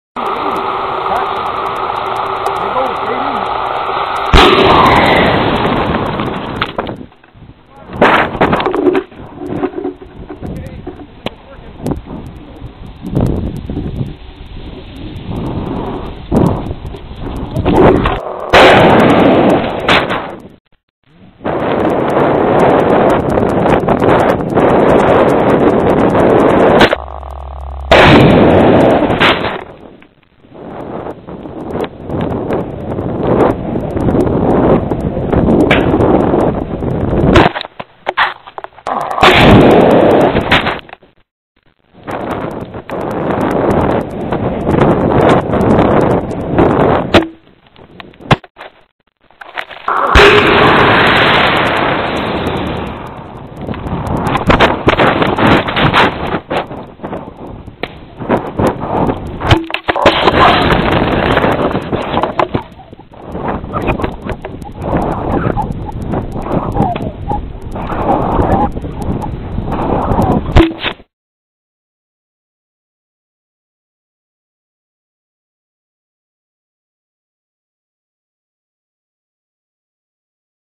A Ride on a Water Rocket
A tiny video camera rides up on AntiGravity's Payload Rocket.  Very reliable launches and parachute recoveries.